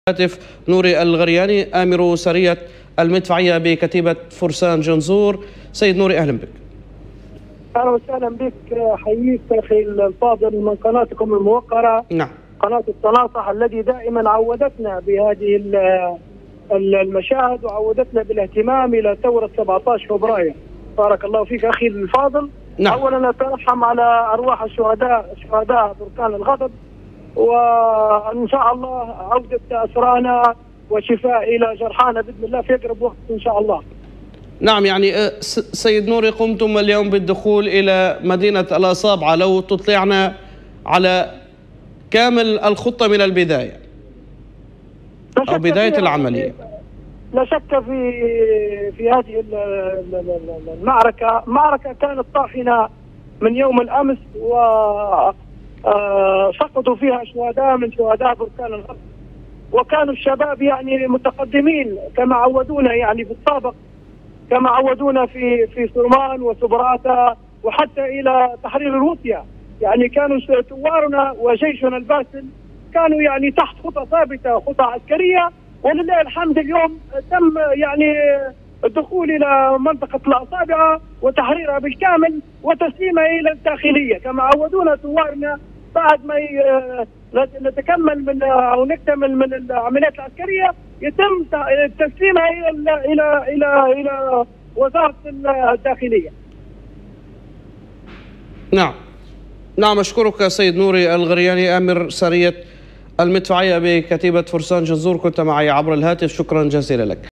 نوري الغرياني/ آمر سرية المدفعية بكتيبة فرسان جنزور: حول عملية تحرير الأصابعة من ميليشيات حفتر
نوري الغرياني/ آمر سرية المدفعية بكتيبة فرسان جنزور: حول عملية تحرير الأصابعة من ميليشيات حفتر | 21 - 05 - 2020

ـــــــــــــــــــــــــــــــــــــــــــــــــــــــــــــــــــــــــ

ــــــــــــــــــــــــــــــــــــــــــــــــــــــــــــــــــــــــــ

ــــــــــــــــــــــــــــــــــــــــــــــــــــــــــــــــــــــــــ

ــــــــــــــــــــــــــــــــــــــــــــــــــــــــــــــــــــــــــ